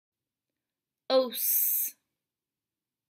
How to pronounce the oʊs sound in American IPA
Learn how to pronounce oʊs sound.
Học cách phát âm chuẩn của âm oʊs trong tiếng Anh-Mỹ. Để học và luyện tập cách phát âm miễn phí, bạn vui lòng truy cập